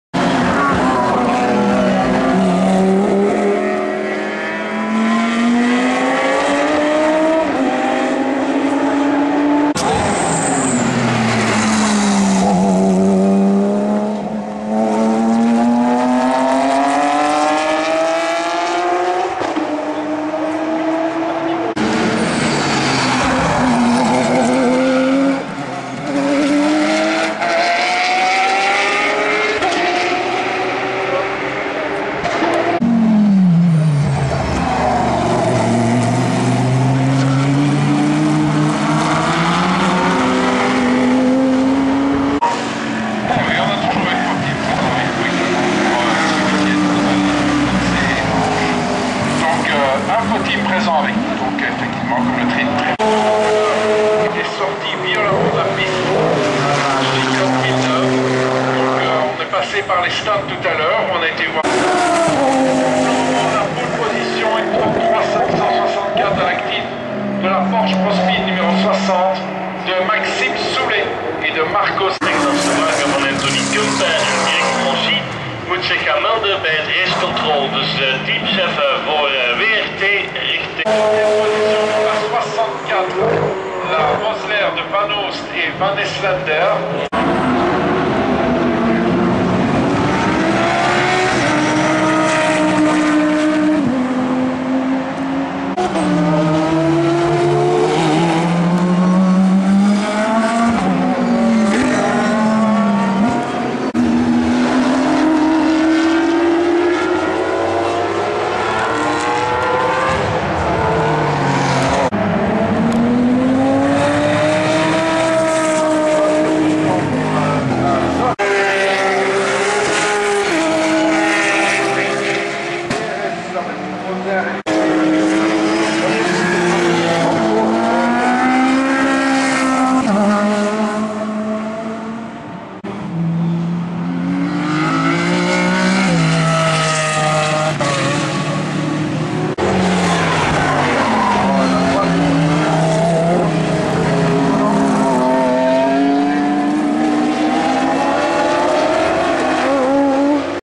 Fia GT3 Zolder 2010 - Belcar
Fia GT3 weekend at Zolder 2010

Qualifying and Free practice at saturday.